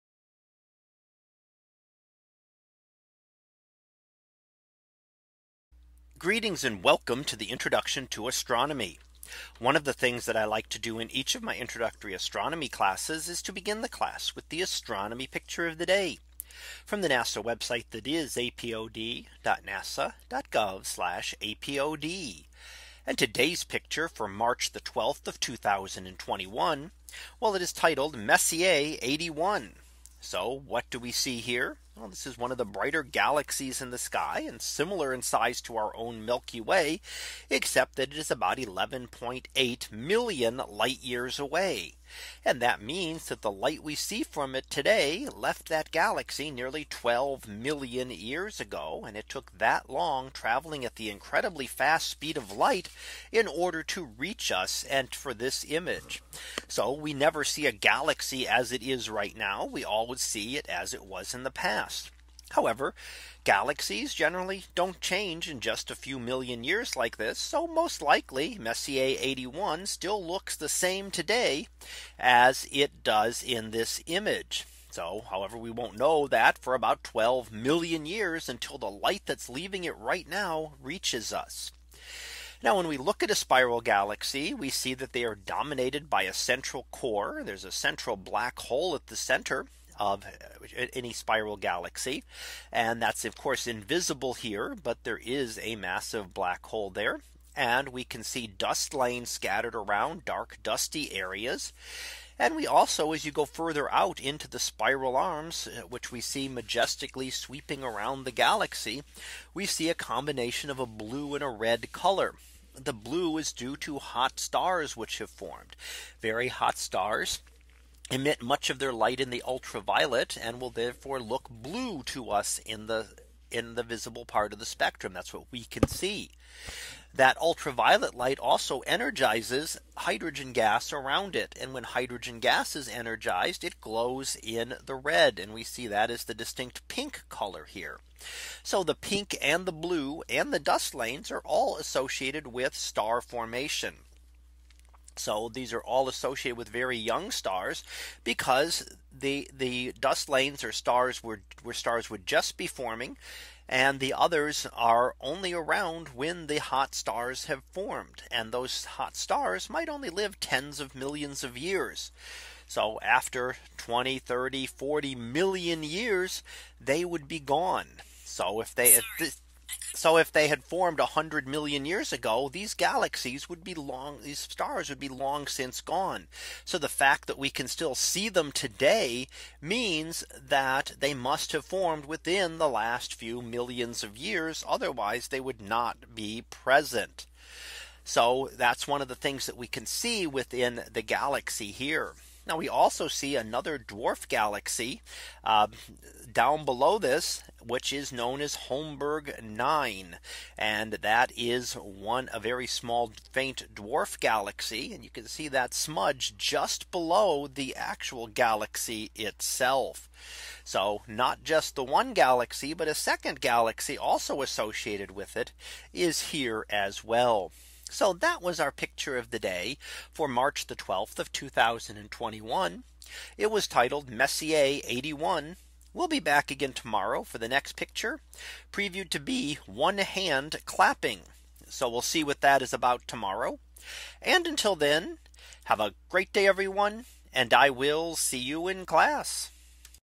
2021 March 12 - Messier 81
In today's image, we see the spiral galaxy known as M 81 or Messier 81. In it, we see the telltale signs of star formation such as dark dust lanes, blue star clusters and red hydrogen regions.